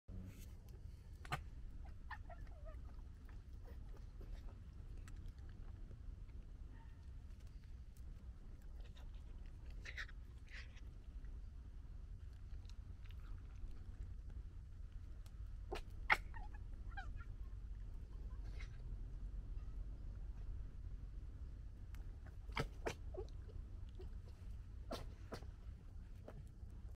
Fox Cub Noisy Eater
Fox Cub#4 and her sister, Fox Cub#6, summer 2021.

Cub#4 was very bold around me and would nip my shoe to ask for treats when she was a bit older. She was the third fox to do this, after Miss Fox and Cub#6. She was the middle of the litter to disperse in winter 2021.

Cub#6 was very bold around me and would sometimes sit under my chair to hide from her siblings. She was the first of 2021's litter of three to disperse in the winter.

Vulpes vulpes, the European Red Fox, is the largest urban wild animal most Brits will see in their lifetimes.

They are the world's most successful and widespread canid.

UK urban foxes only began to colonise our towns and cities in the 1930s.

Recorded using a DJI Osmo Pocket 2 with extension handle.

Don't forget to enable notifications for the latest fox uploads!